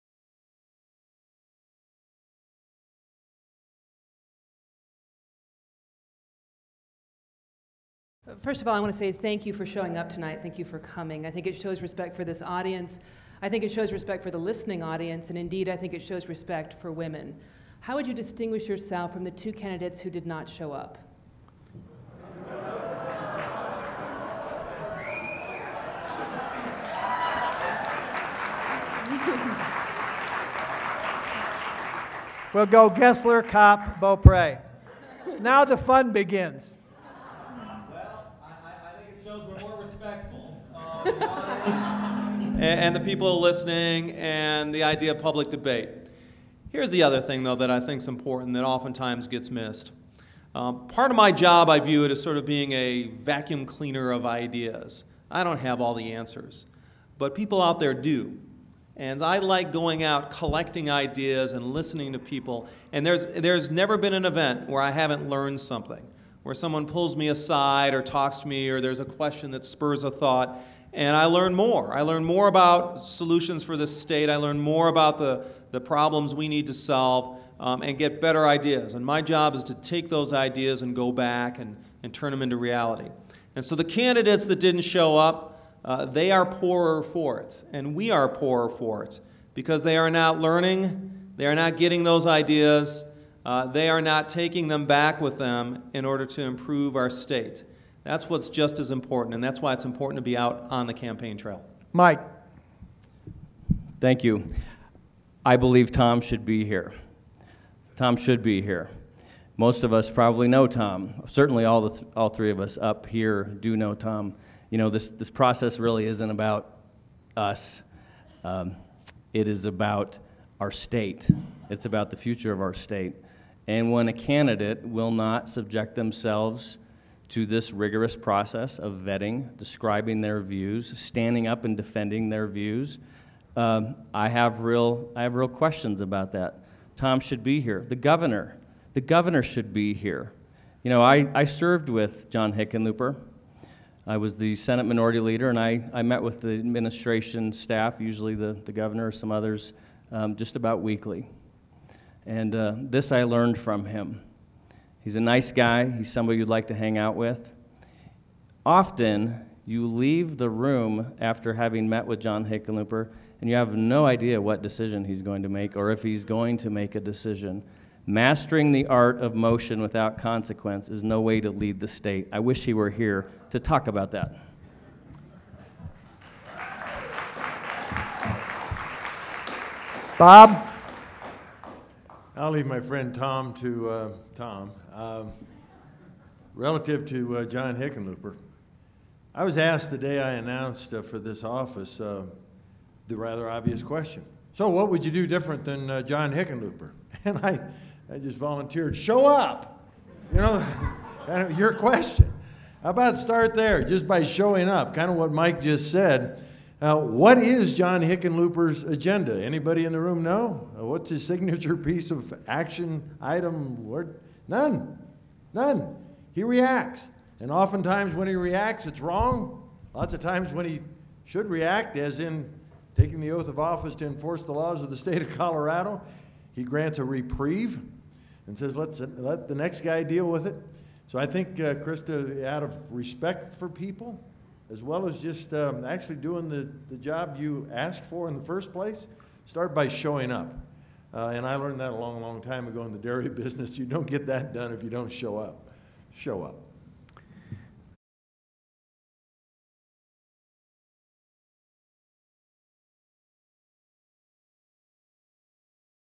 How Colorado Gubernatorial Cadidates are Different
At the Colorado 2014 Gubernatorial Debate hosted by Centennial Institute, the three present gubernatorial candidates (Mike Kopp, Scott Gussler, and Bob Beauprez) explain how they are different from the other two candidates, John Hickenlooper and  Tom Tancredo who did not show up for the debate.